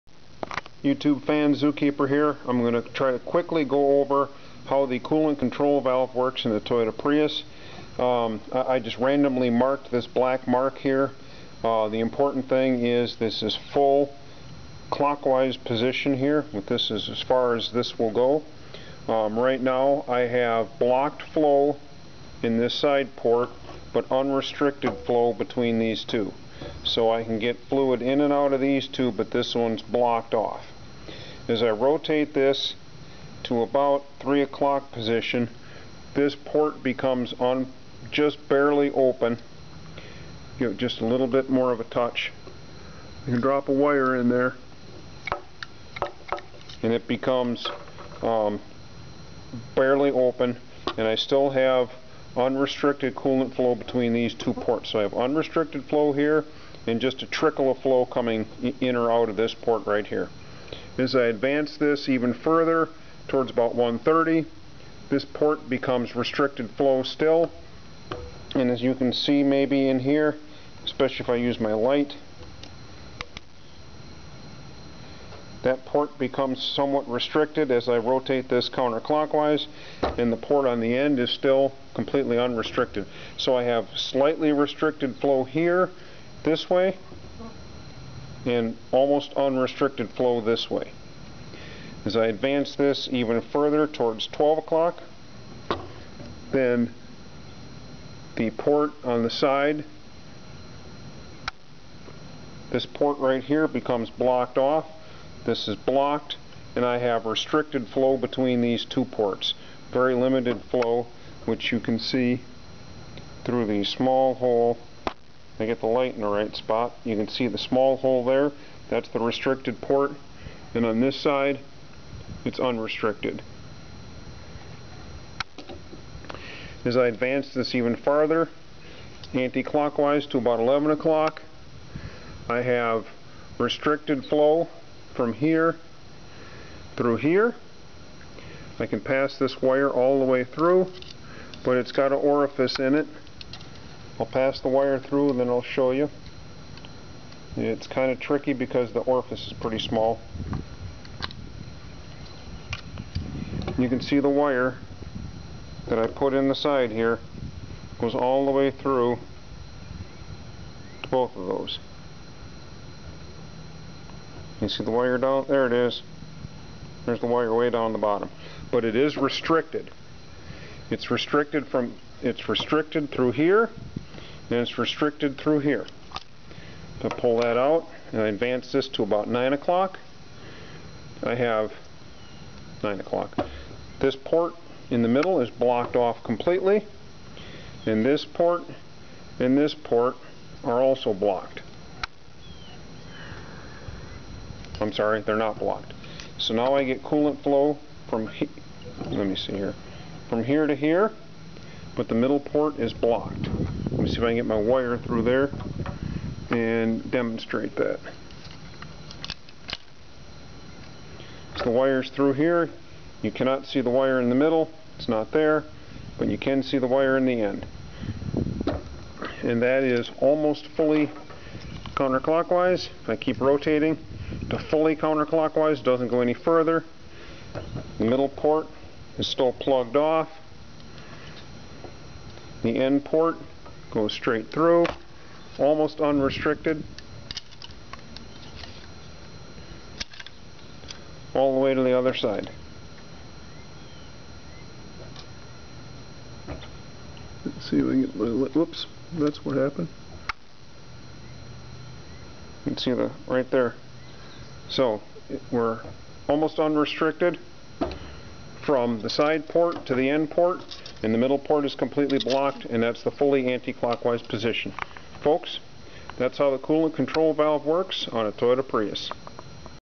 Gen2 Prius Coolant Control Valve - Coolant Flow Paths
How the coolant flows thru a Gan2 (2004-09) Toyota Prius.